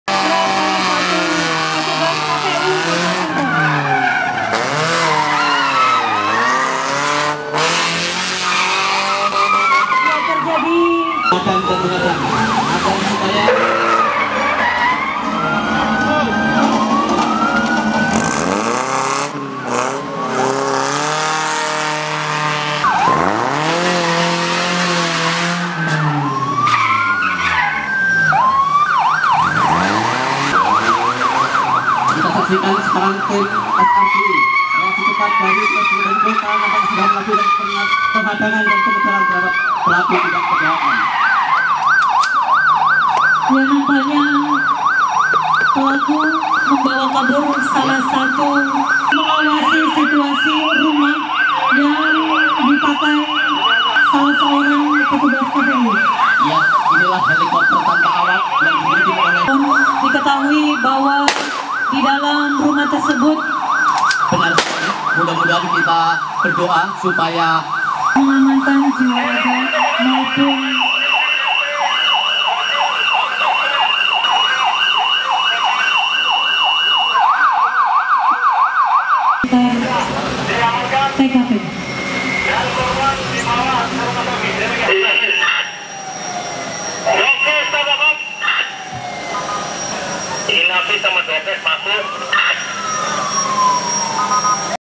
Ngepot mobil polisi tangkap penjahat di cirebon
simulasi pengamanan pilkada serentak 2018, dilakukan polresta cirebon, jawa barat. sebanyak 1500 personil gabungan dilibatkan